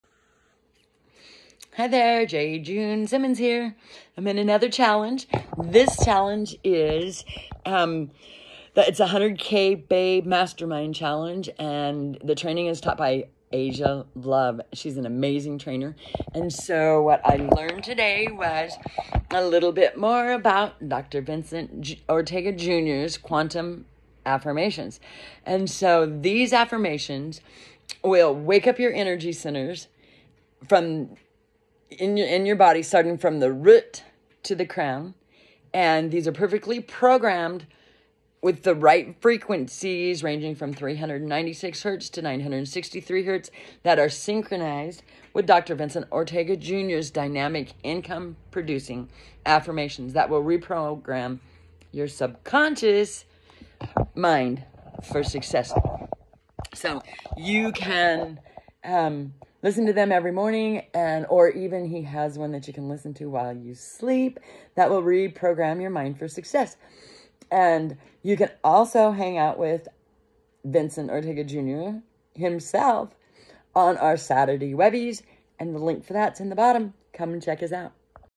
Babes Club 100K Mastermind
how Quantum affirmations can help you transform your mind for more success